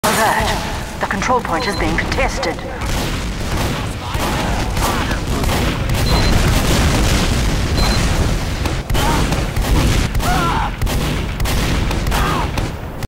Replay plus competetive = win
First time I played a scrim with replay on. Got a sexy airshot :3. I doubt I'll post many comp replays due to the lack or replay supporting servers... but sub if you like pub stuff :D